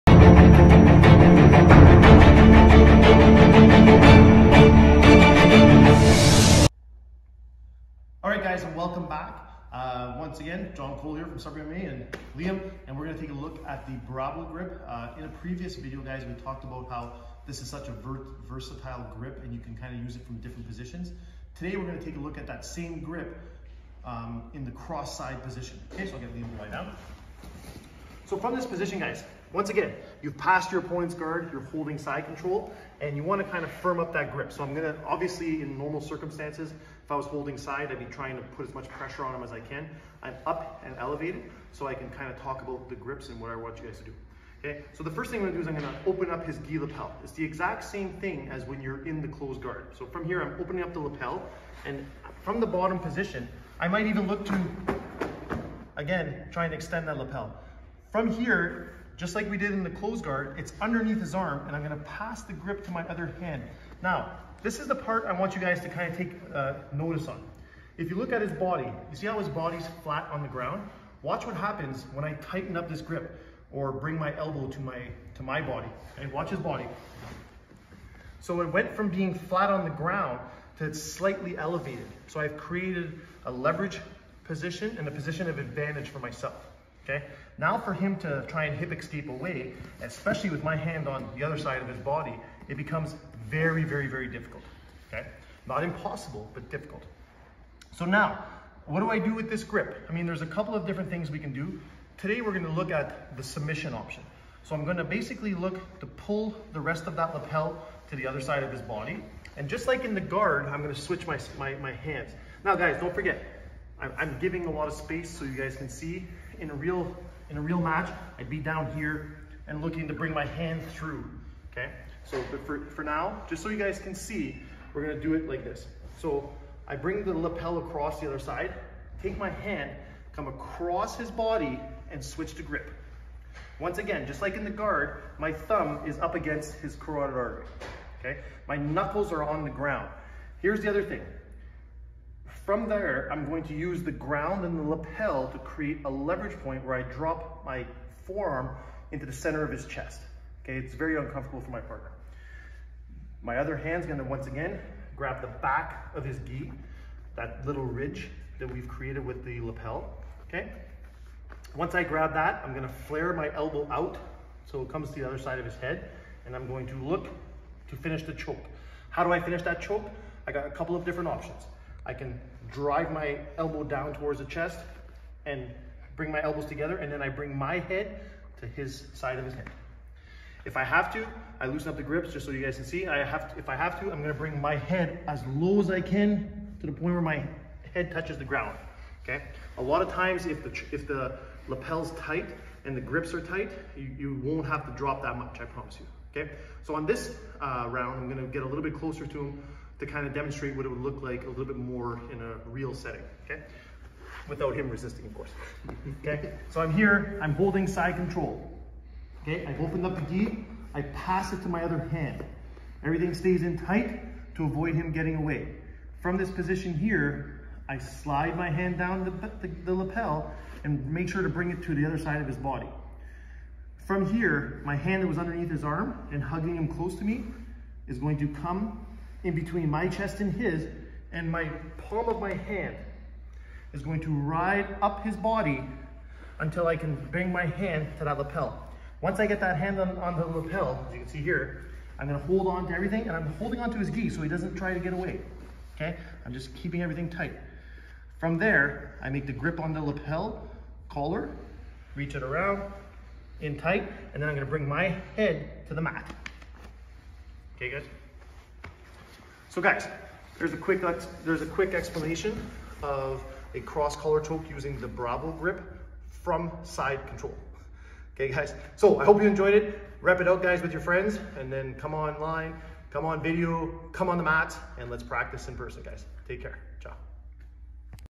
Cross Collar Choke - From Brabo Grip
In this video, we review the cross collar choke using the Brabo Grip from Side control.